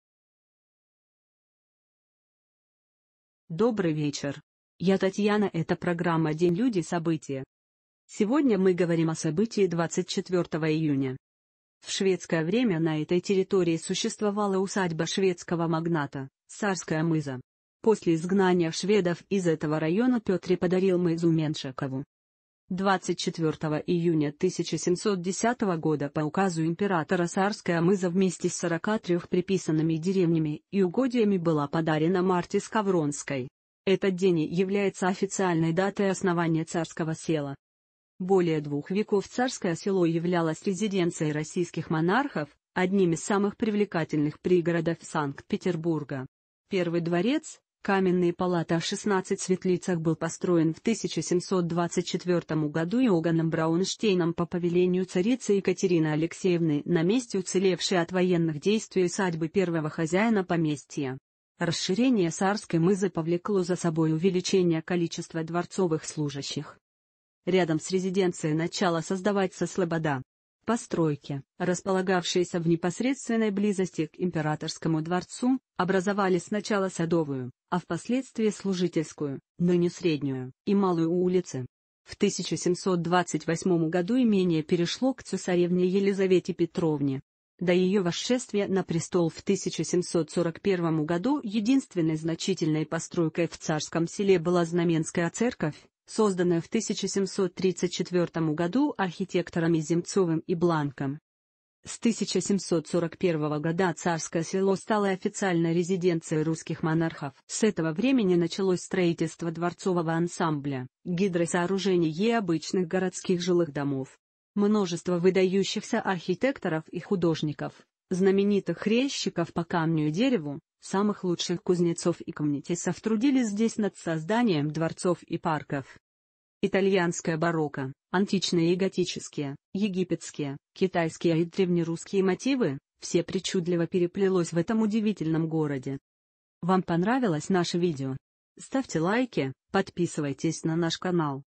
День Люди События.24 июня.Основано Царское село близ Санкт-Петербурга
Электронная книга «Раскрутка на YouTube»

Помощь каналу День Люди События.24 июня.Основано Царское село близ Санкт-Петербурга
В шведское время (1609—1702) на этой территории существовала усадьба шведского магната — Сарская мыза. После изгнания шведов из этого района Петр I подарил мызу А.Д. Меншикову.

24 июня 1710 года по указу императора Сарская мыза вместе с 43 приписанными деревнями и угодьями была подарена Марте Скавронской (ставшей в 1712 году его женой под именем Екатерины Алексеевны). Этот день и является официальной датой основания Царского Села.

Более двух веков Царское Село являлось резиденцией российских монархов, одним из самых привлекательных пригородов Санкт-Петербурга. Первый дворец, «каменные палаты о 16 светлицах» был построен в 1724 году Иоганном Браунштейном по повелению царицы Екатерины Алексеевны на месте уцелевшей от военных действий усадьбы первого хозяина поместья. Расширение Сарской мызы повлекло за собой увеличение количества дворцовых служащих.

Рядом с резиденцией начала создаваться слобода. Постройки, располагавшиеся в непосредственной близости к Императорскому дворцу, образовали сначала Садовую, а в последствии Служительскую (ныне Среднюю) и Малую улицы. В 1728 году имение перешло к цесаревне Елизавете Петровне. До ее восшествия на престол в 1741 году единственной значительной постройкой в Царском Селе была Знаменская церковь, созданная в 1734 году архитекторами М.Земцовым и И.Бланком.

С 1741 года Царское Село стало официальной резиденцией русских монархов. С этого времени началось строительство дворцового ансамбля, гидросооружений и обычных городских жилых домов. Множество выдающихся архитекторов и художников, знаменитых резчиков по камню и дереву, самых лучших кузнецов и камнетесов трудились здесь над созданием дворцов и парков.

Итальянское барокко, античные и готические, египетские, китайские и древнерусские мотивы – все причудливо переплелось в этом удивительном городе.

День Люди События 24июня ОснованоЦарскоеселоблизСанкт-Петербурга#ТВИНФО#АлександрЛебедев

Основано Царское село близ Санкт-Петербурга Наш новый канал новостей и технологий ТВ -Инфо